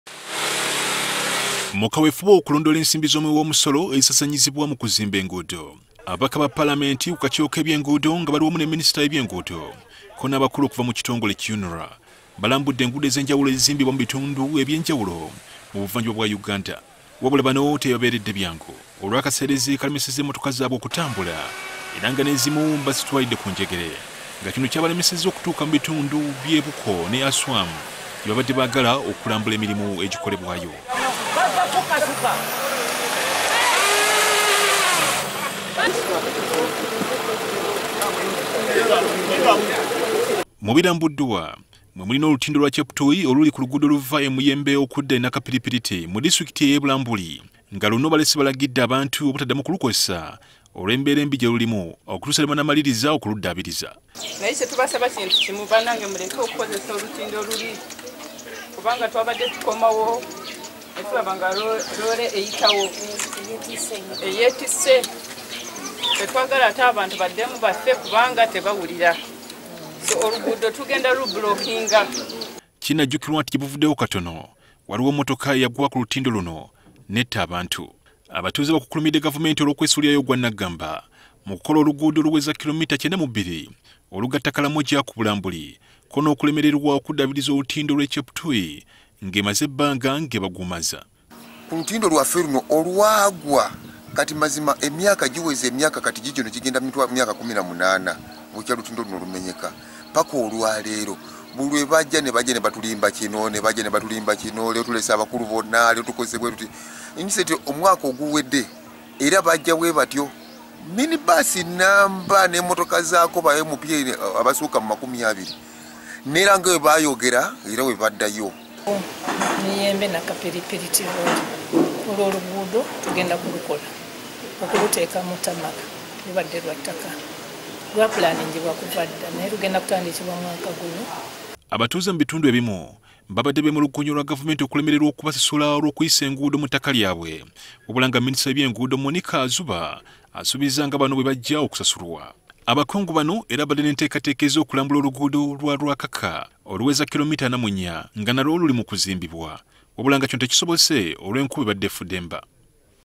ENGUUDO EMBI: Motoka z’ababaka ne minisita zitubidde
Ababaka ku kakiiko ka palamenti akalondoola eby’enguudo basanze akaseera akazibu, mmotoka zaabwe bwezitubidde olw’akaseerezi bwebabadde balambula emirimu egyikolebwa ekitongole ky’ebyenguudo ki UNRA mu buvanjuba bw’eggwanga.

Bano nga bali wamu n’abakungu ba UNRA ko ne minisita w’ebyenguudo, baggadde olutindo oluyitibwa Cheptui, nga kigambibwa nti luno lubadde lufuuse kattiro.